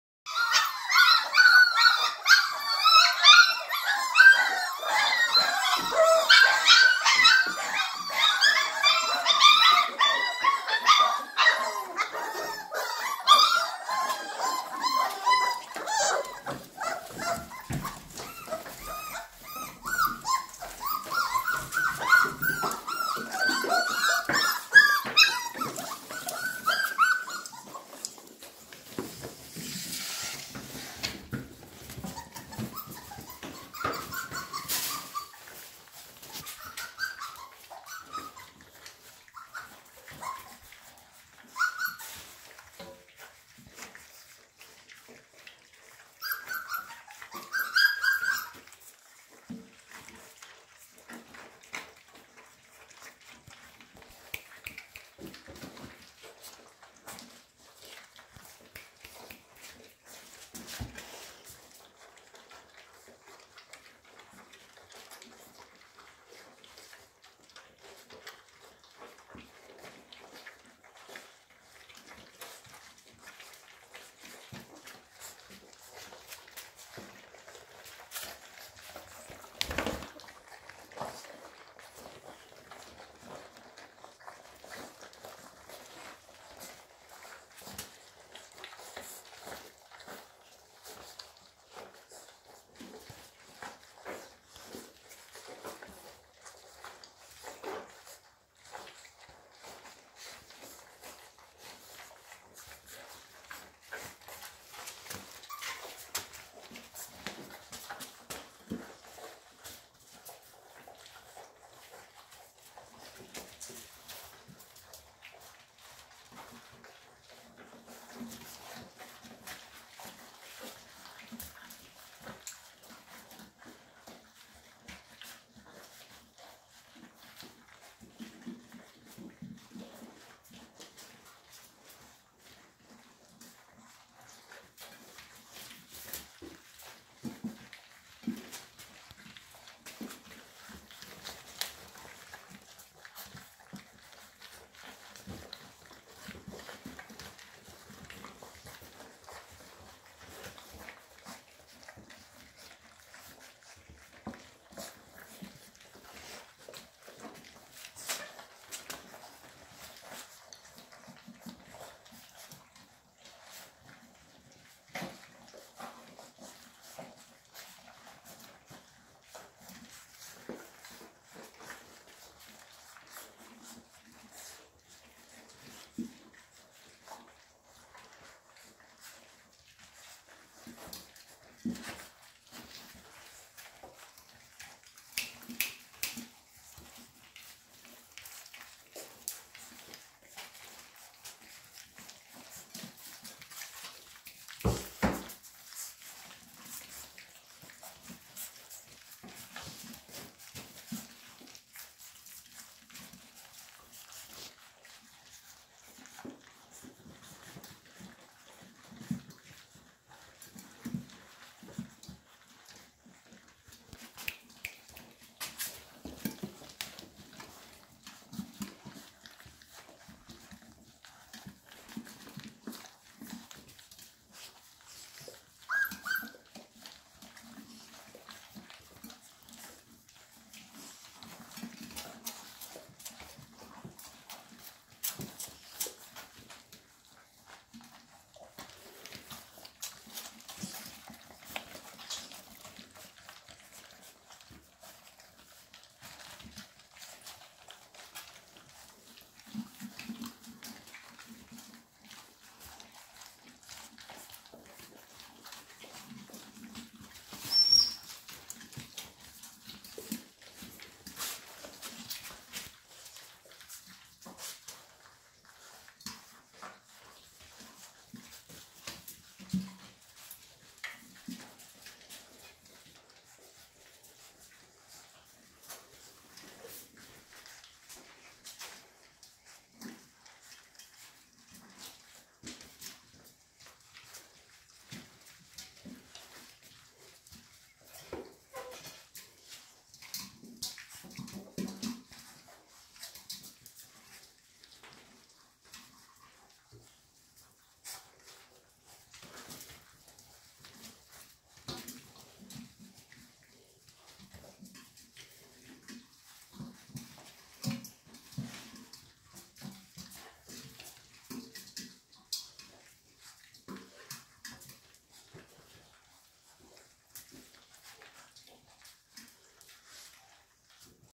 Litter V - Feeding Frenzy
When I first come into the puppy cottage, all is calm and quiet and the when the door closes the pups are usually roused from sleep. Once the dinner pans start clinking the pups GO CRAZY! I have to put in my ear plugs just so I can stand being in the room to finish preparing the weaning meal. Here's a glimpse into the routine and how the pups react.  Once the flood gates are open look out! The pups come running out, find the food bowl and literally GO TO TOWN! Thankfully it gets quiet again while they are chowing down.  Once the weaning meal is gone they pursue momma for a milkbar dessert, however she's not having it.  She is also helping with the weaning process by not allowing them to nurse. -- Video taken 02/28/2019 @10:54 PM